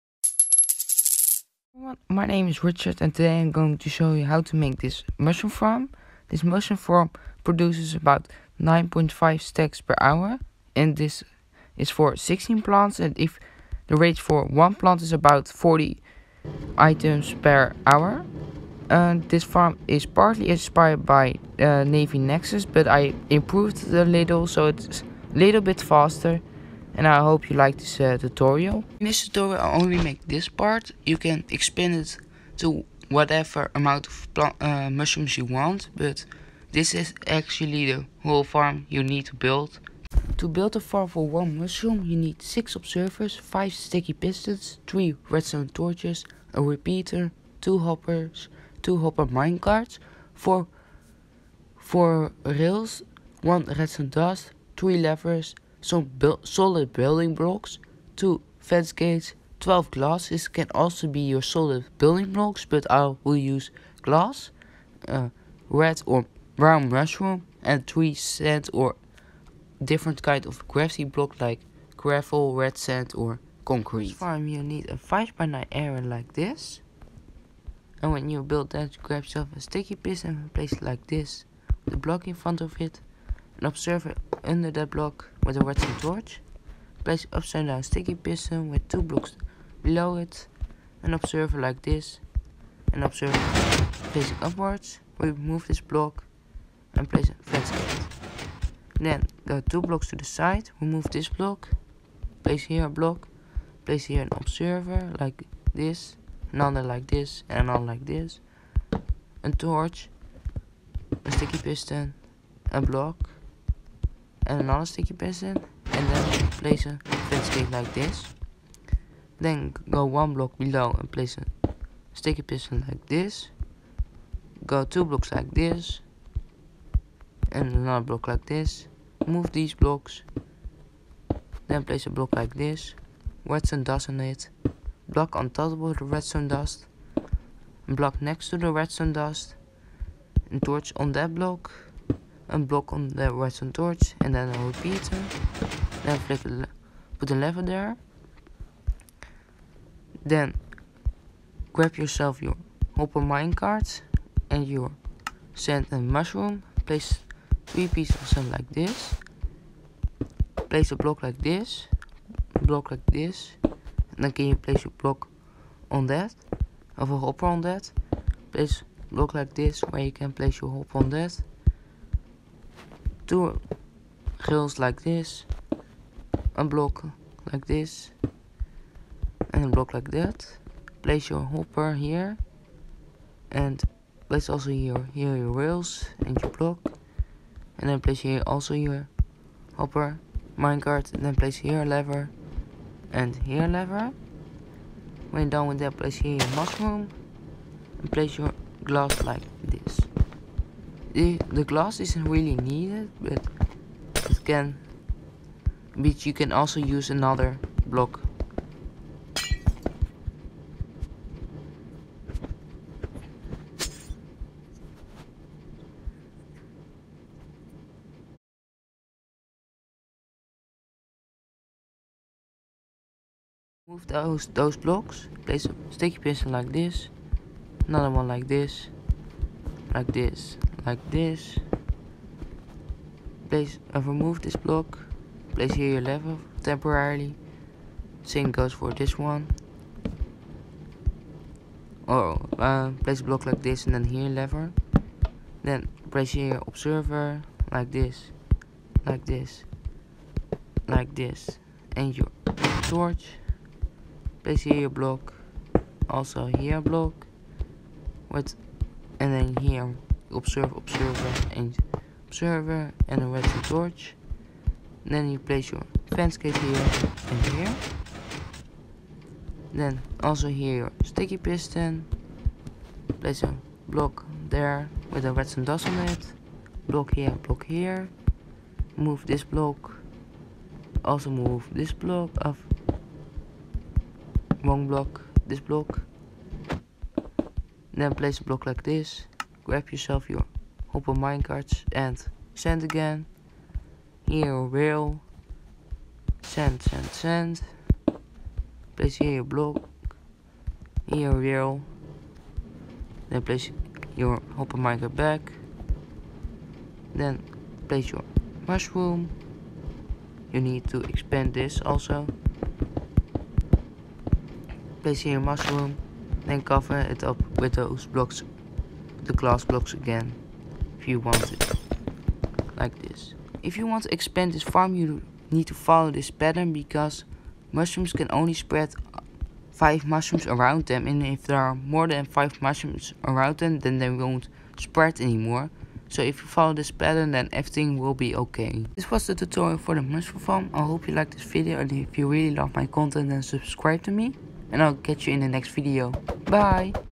[Broken in 1.17+] 0-tick Mushroom farm
Hello everyone, in this video I made a 0-tick Mushroom farm

Rates:
-16 mushroom = 9.5 mushrooms/hour
-1 mushroom = ~40 mushrooms/hour

NavyNexus video about his 0-tick Mushroom farm (he also has a good explanation about the mushroom mechanics)